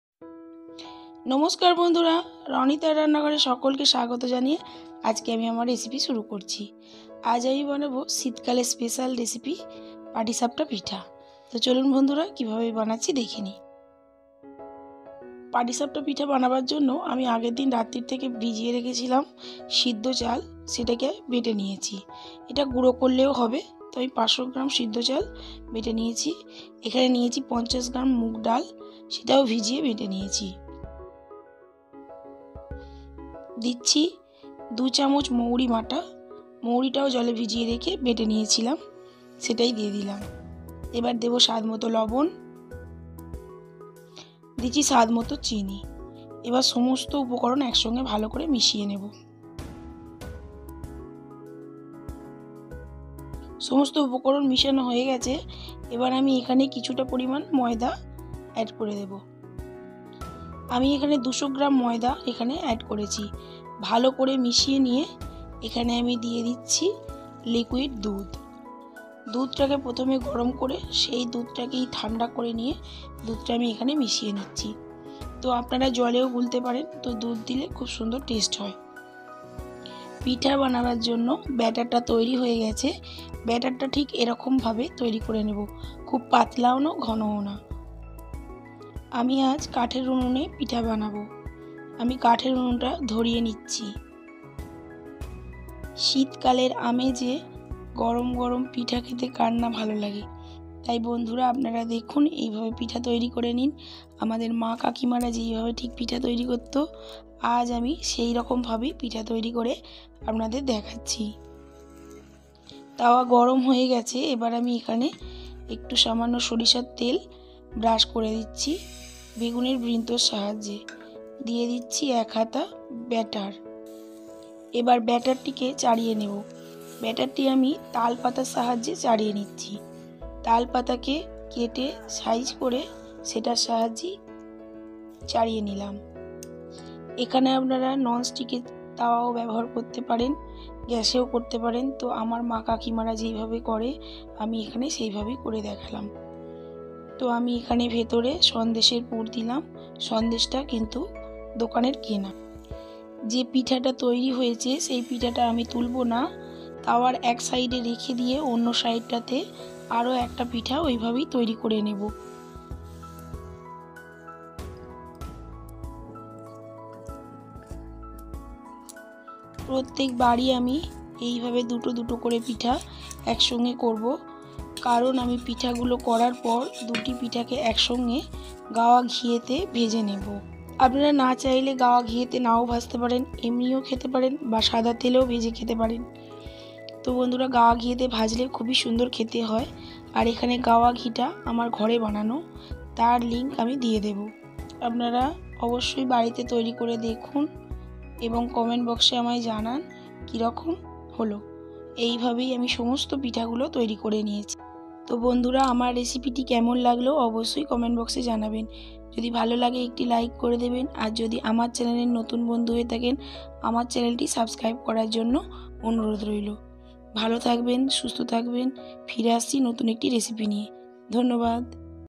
ঘরোয়া পদ্ধতিতে শীত কালের স্পেশাল রেসিপি/ Popular Bengali Patishapta Pitha recipe/Ranitar Ranna Ghar
Hi everyone welcome to my chenel Ranitar Ranna Ghar that video is Patishapta Pitha recipe  Bengali traditional recipe  Bengali dessert recipe